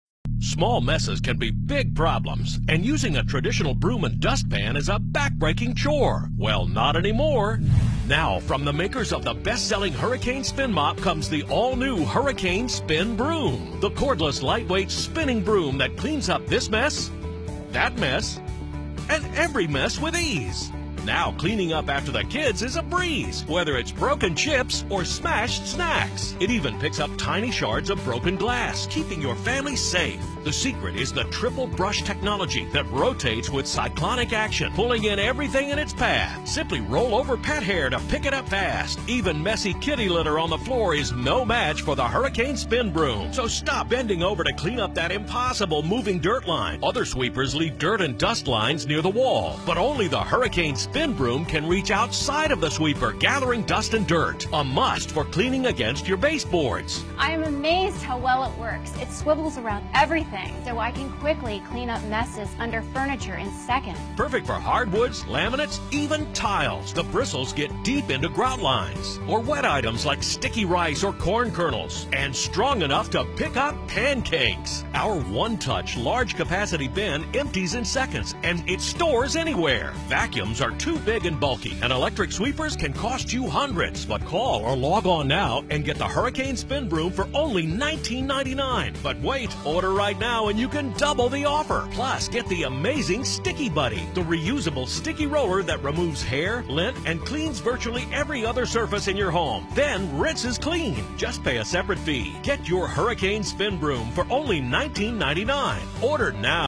Turbo Tiger Sweeper
Made of stainless steel, it is durable.
It has a handle with adjustable length.
Easy driving pole, very convenient cleaning.
Brush degree with high speed to drop garbage
Suitable for almost all kinds of floor, such as wooden floor, pvc address, marble, ceramic address, cement land, etc.
Specifications:
Material: Plastic, Steel, Steel
Size: 385x215x93
Color: Blue and White
Roll Package:
1 x sweeper
3 x slide rod
1 x square
1 x manual
Notes:
1. Due to the difference between different monitors, the picture may not reflect the color of the actual item. We guarantee the style is the same
2. Because of manual measurement and different measurement methods, please allow 1-3 centimeter deviation. Thanks